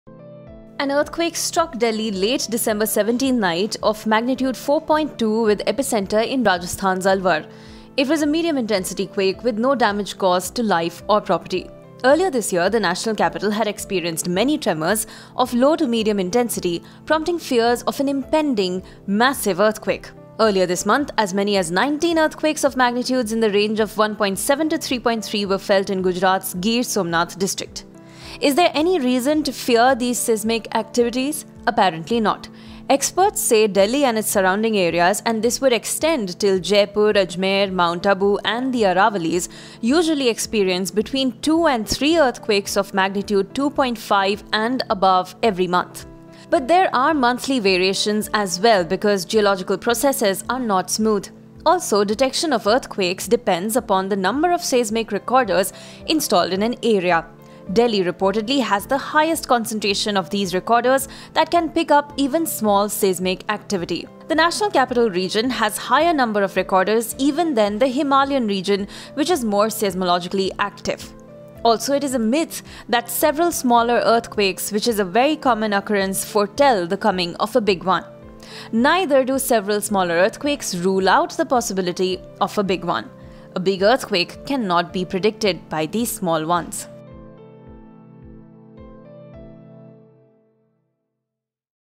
Delhi earthquake: Is a big one coming after several small tremors? | Oneindia News
An earthquake struck Delhi late December 17th night of magnitude 4.2 with epicentre in Rajasthan's Alwar. No damage was caused to life or property. Earlier this year, the national capital had experienced many tremors of low to medium intensity prompting fears of an impending massive earthquake. Is this a possibility?